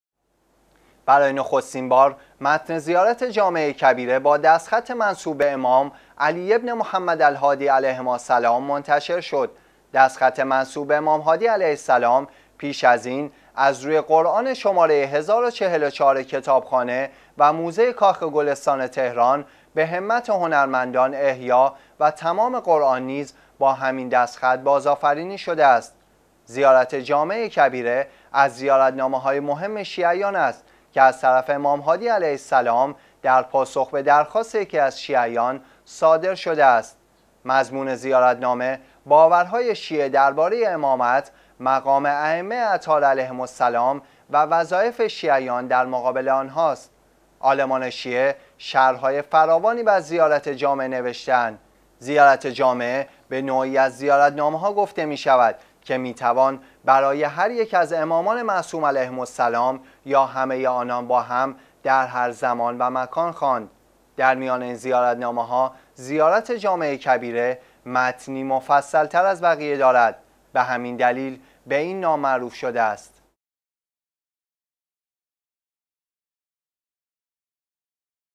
زیارت جامعه کبیره با دستخط منسوب به امام هادی علیه السلام منتشر شد
برای نخستین بار متن زیارت جامعه کبیره با دستخط منسوب به امام علی بن محمد الهادی علیهماالسلام منتشر شد.
دستخط منسوب به امام هادی علیه السلام پیش از این از روی قرآن شماره ۱۰۴۴ کتابخانه و موزه کاخ گلستان تهران به همت هنرمندان احیا و تمام قرآن نیز با همین دستخط بازآفرینی شده است.
زیارت جامعه کبیره از زیارتنامه‌های مهم شیعیان است که از طرف امام هادی علیه السلام در پاسخ به درخواست یکی از شیعیان صادر شده است.
مضمون زیارتنامه باور‌های شیعه درباره امامت، مقام ائمه اطهار علیهم السلام و وظایف شیعیان در مقابل آن‌هاست.
عالمان شیعه، شرح‌های فراوانی بر زیارت جامعه نوشته‌اند.
زیارت جامعه به نوعی از زیارتنامه‌ها گفته می‌شود که می‌توان برای هر یک از امامان معصوم علیهم السلام یا همه آنان با هم، در هر زمان و مکان خواند. در میان این زیارتنامه‌ها «زیارت جامعه کبیره» متنی مفصل‌تر از بقیه دارد؛ به همین دلیل به این نام معروف شده است.